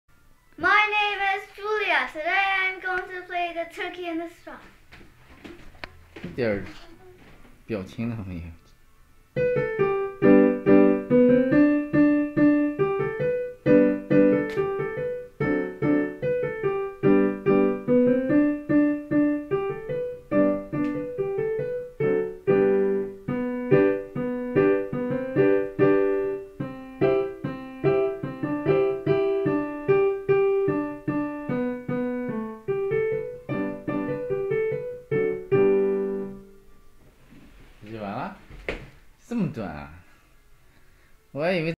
大宝弹钢琴
大宝弹琴练习首次表演的曲目。